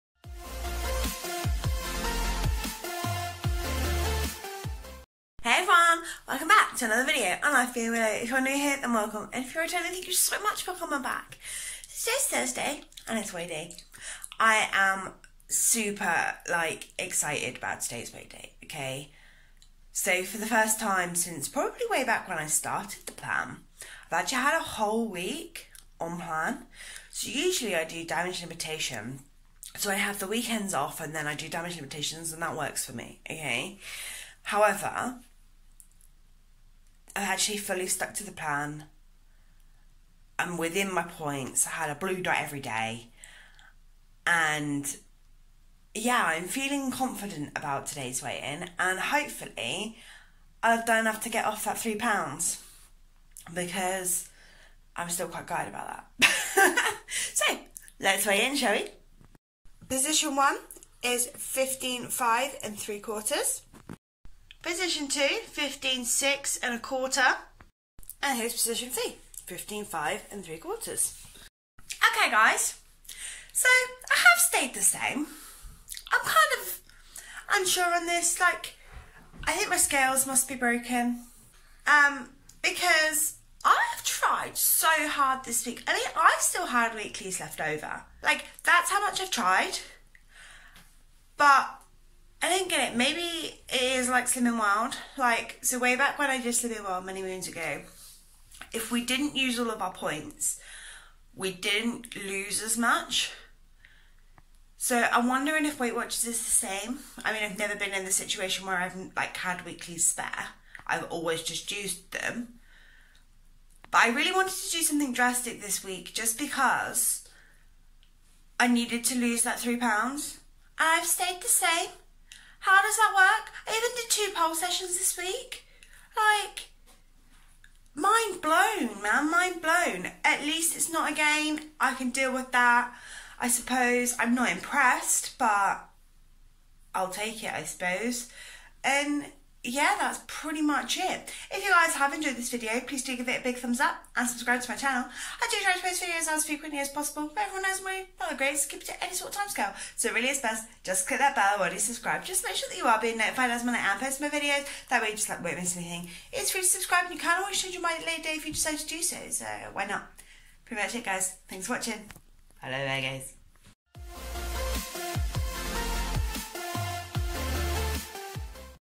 Weigh day 21/01/21
S/W 20st 8lbs
C/W 15st 6lbs
Following weight watchers blue plan
Total loss 5st 0.5lbs
Stayed the same at Today's weigh in.

Follow me on Connect - willow.wren